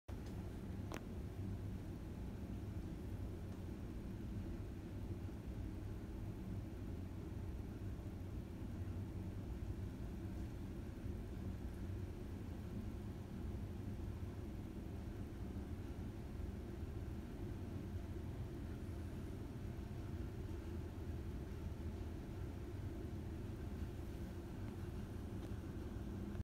Cabo Sand Spider
Found this little guy crawling the ceiling.